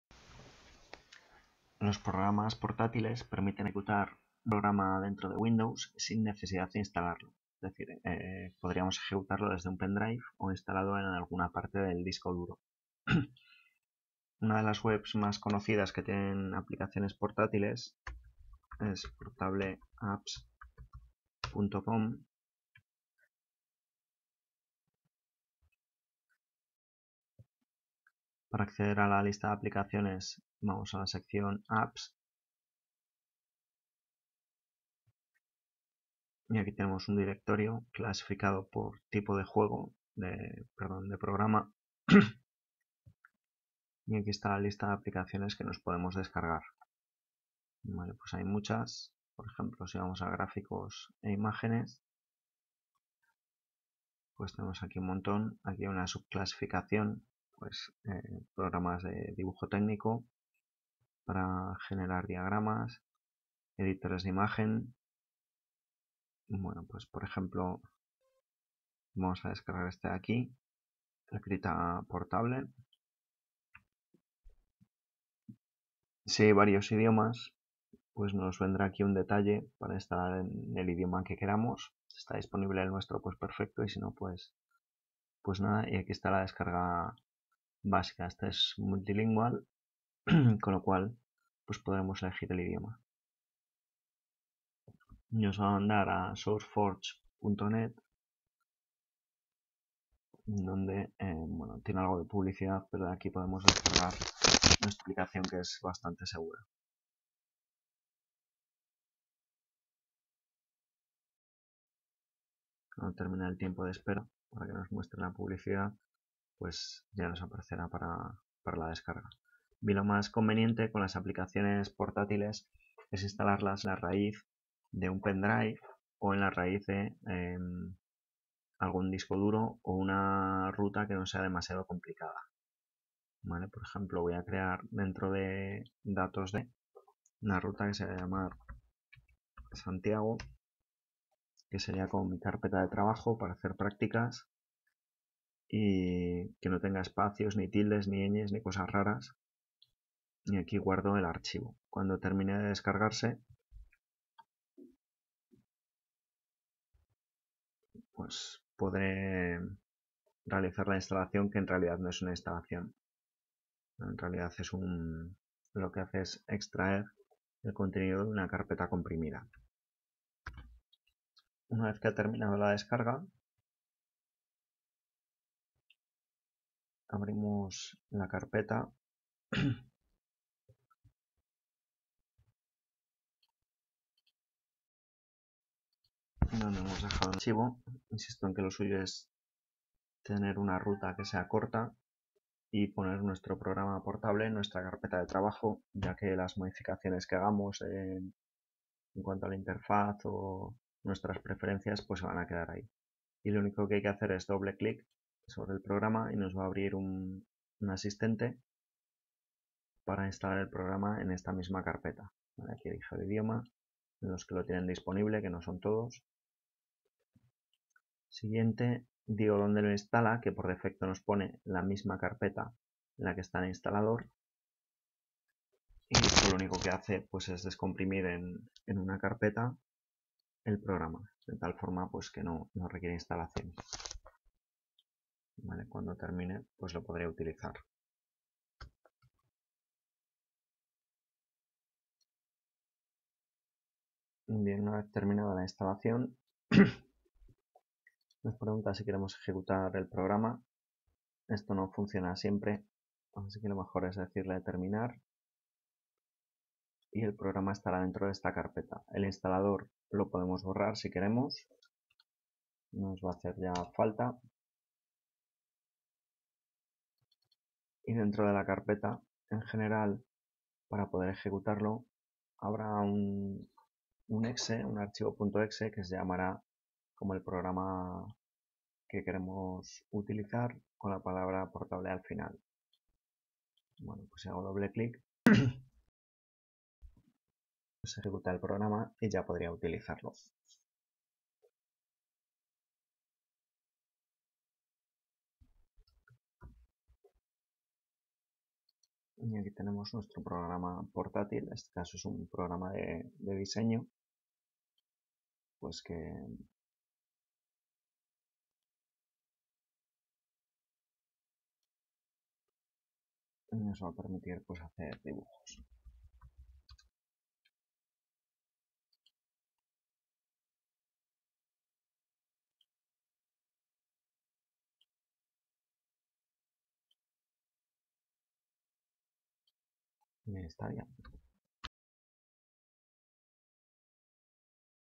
Instalar Programas portátiles en Windows
Cómo instalar correctamente un programa portátil (portable) en Windows. La web de referencia que se utiliza es PortableApps.com